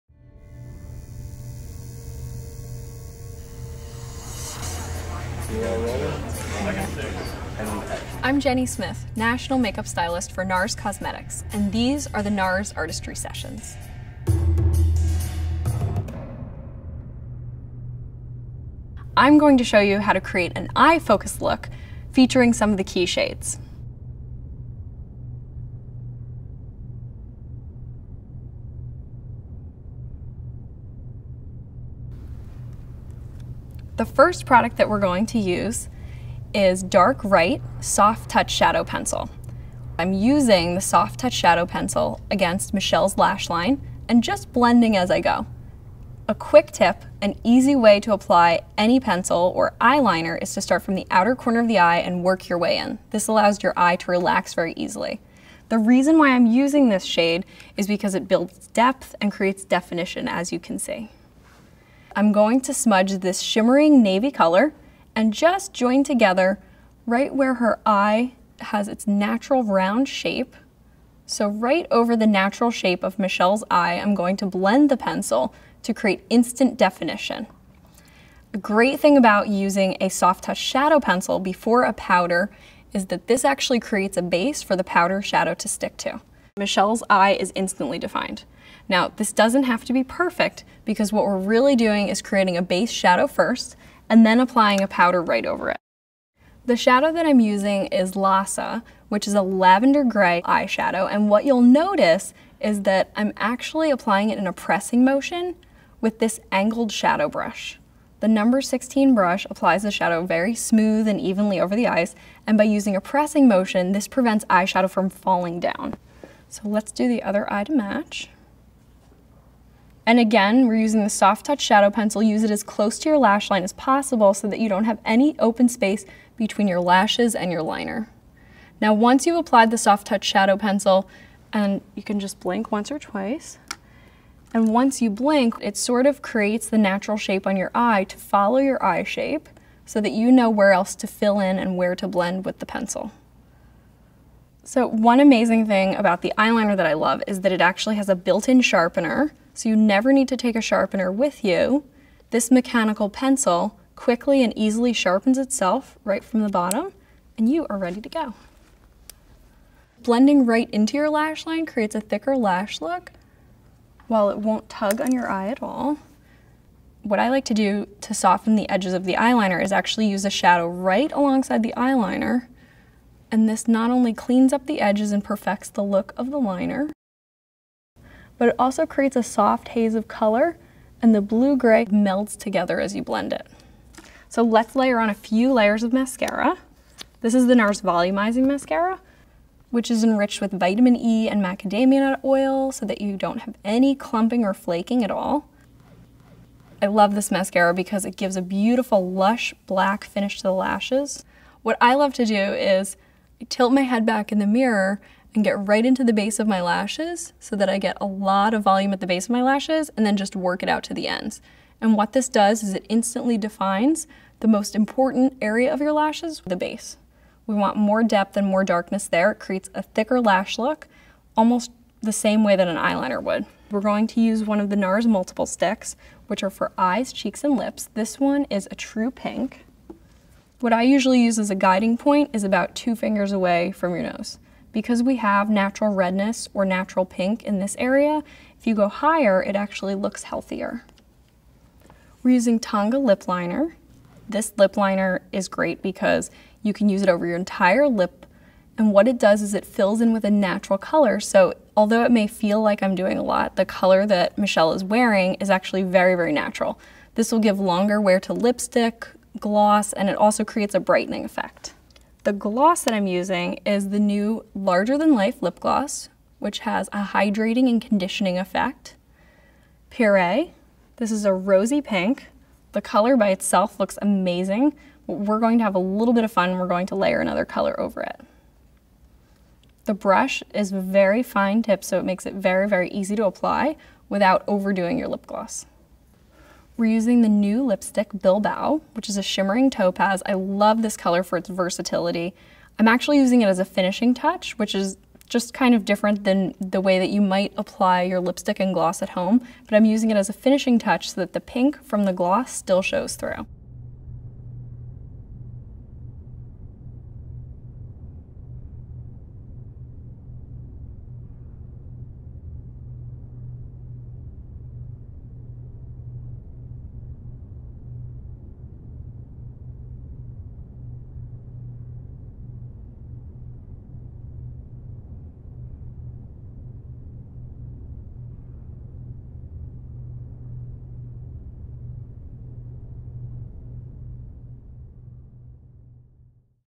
NARS How To: Spring 2012 Color Collection - Eyes
The Spring 2012 Collection, created by Founder and Creative Director François Nars, stars a mix of bright and playful colors balanced with rich, saturated hues. Eyes glisten in muted tones of lavender and pink, or shimmer in bronze shades accented with rich navy shadow pencil. Cheeks are flushed with candy pink. Lips gleam in topaz or pop with berry. Metallic violet nails add the finishing touch to a collection that's bright, modern and iconic.

PRODUCTS USED